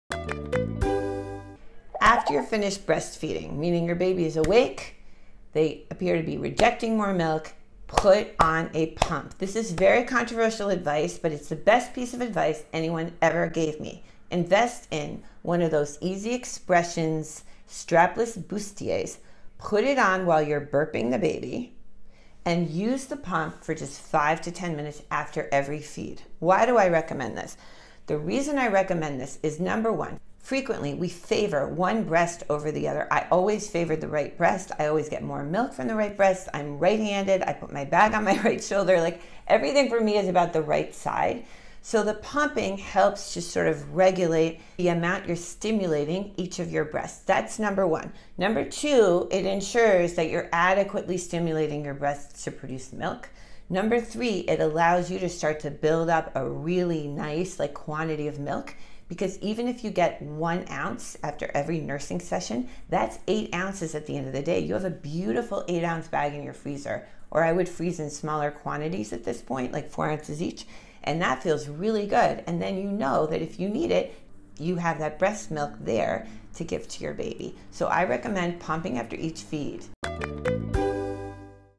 1-Week-Old-Baby: Newborn Pumping and Breastfeeding Schedule | Subt. ENG/ FR/ ES/ ZHO_CN | Cloudmom
Find more about your 1 week old baby Wondering "How much should I be pumping 1 week postpartum"? Watch this video for tips on using the breast pump after each nursing session and for a breastfeeding and pumping schedule for a 1-week-old baby.